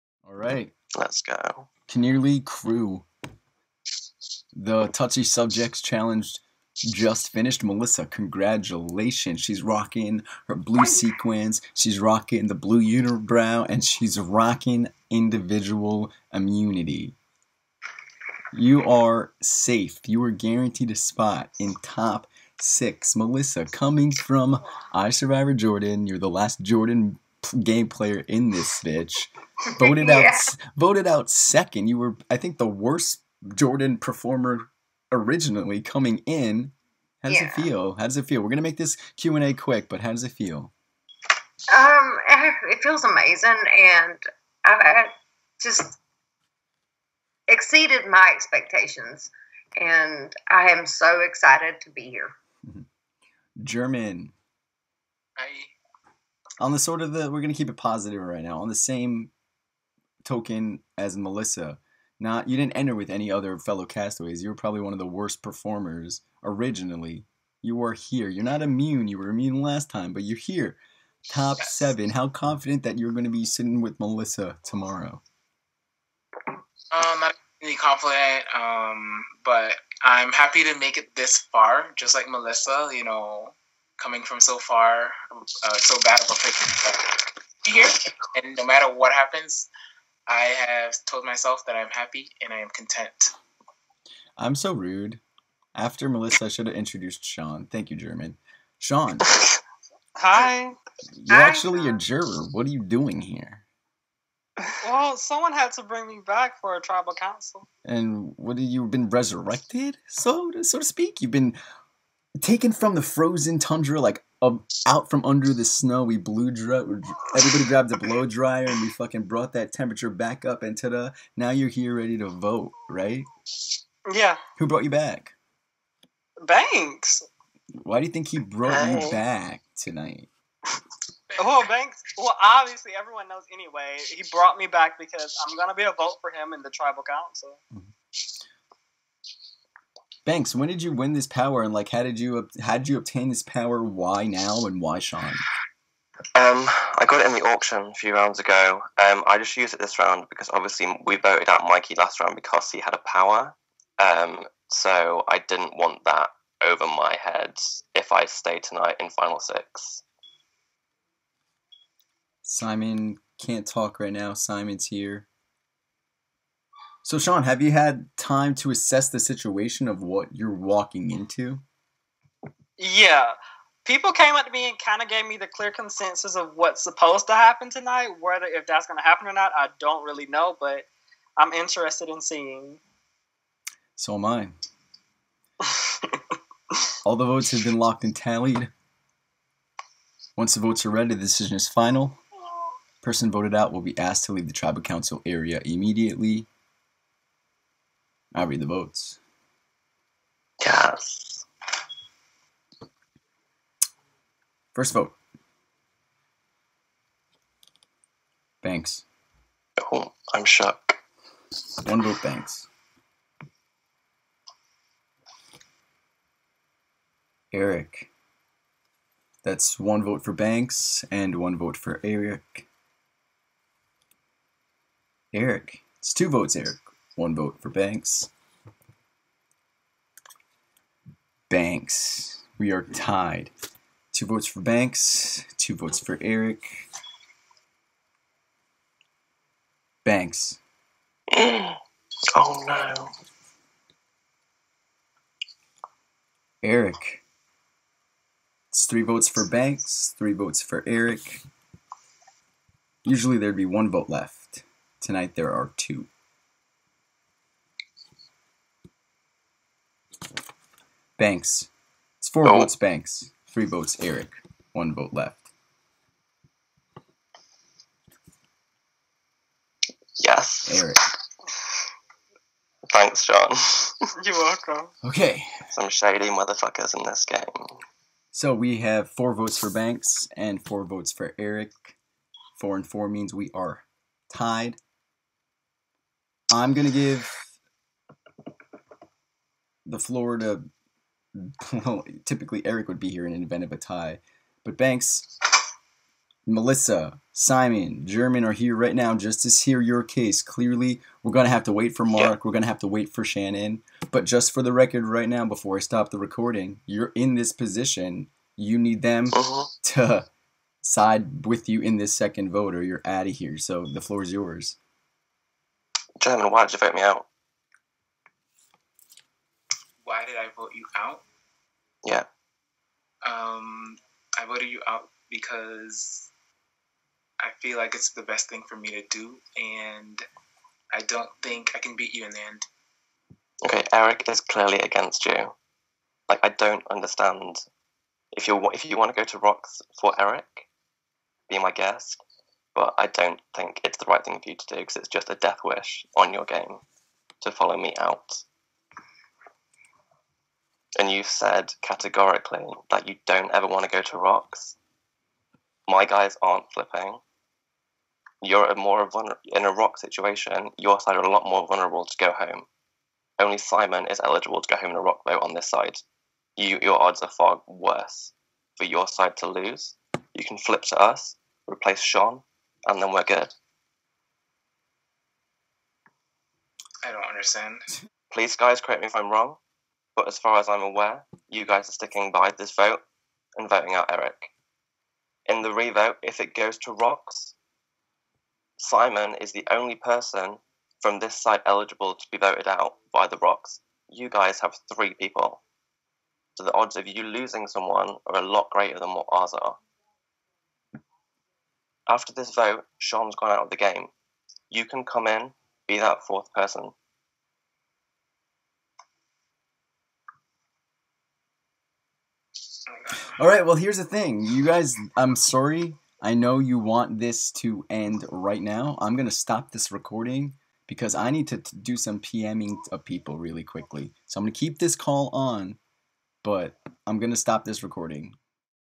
iSurv1vor 31: Queen Elizabeth Islands (Tribal #16/ Qinirli Crew)
Portion of this TC was non-live because of scheduling conflicts, this was the revealing of results.